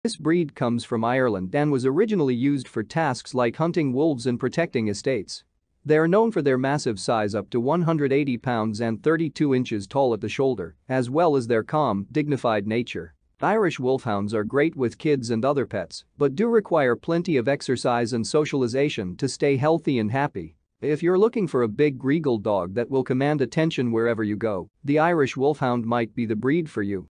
Irish Wolfhound
Discovering the Majestic and Mighty Irish Wolfhound: A Breed Steeped in History and Legend
In this short video, we introduce you to the Irish Wolfhound, a majestic and mighty breed of dog that has captured the hearts of many dog lovers. Watch as we showcase some of the unique characteristics of this magnificent breed, including their large size, shaggy coat, and gentle and noble personality. From their origins as a hunting and fighting dog in ancient Ireland to their popularity as a companion and show dog today, the Irish Wolfhound has a rich and fascinating history.